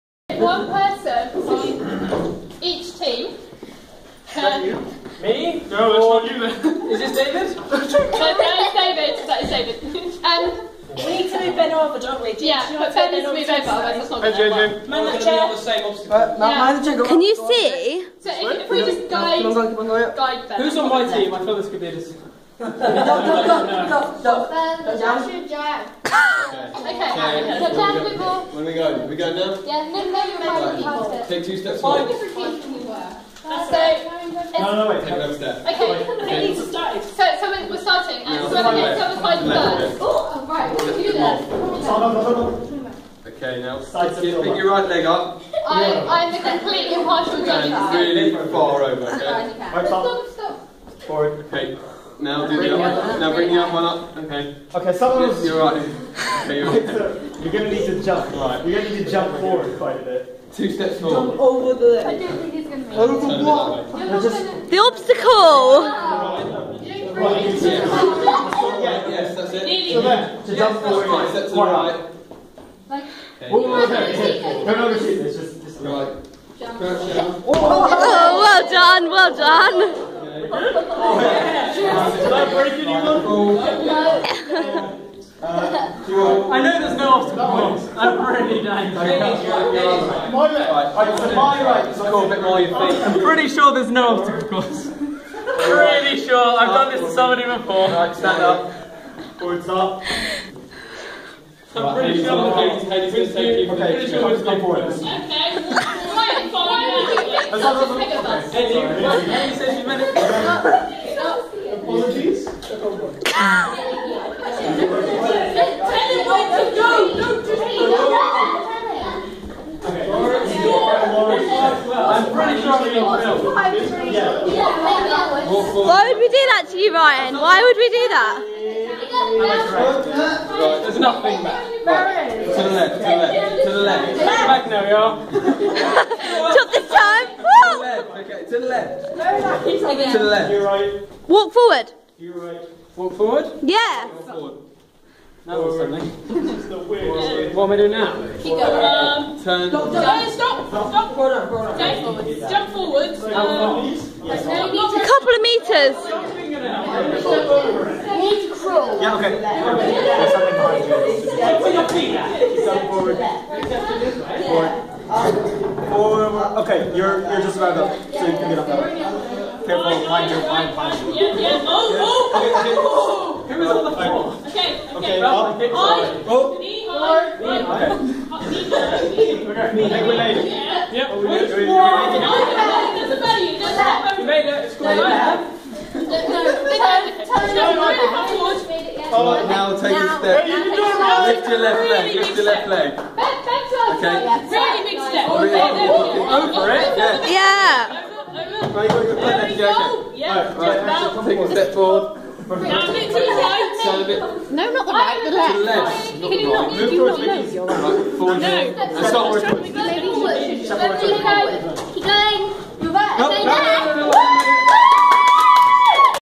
Guiding blindfolded people around no obstacles!
Told them that leading round a obstacle course of people but there wasn't really any 😂